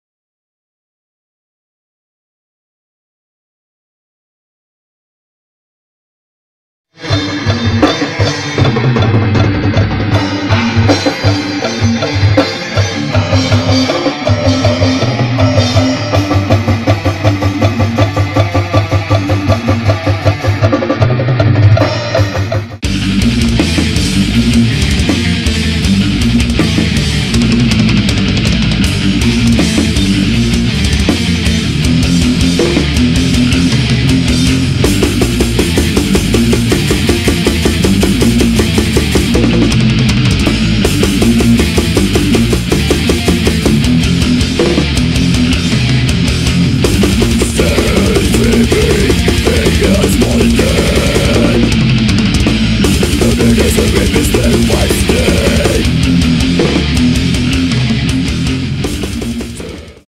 Hadeath Records x Gorerock
Teaser 1
Cuplikan salah satu lagu kami yang akan dirilis dalam bentuk CD bulan Agustus mendatang oleh Hadeath Records, nantikan update selanjutnya.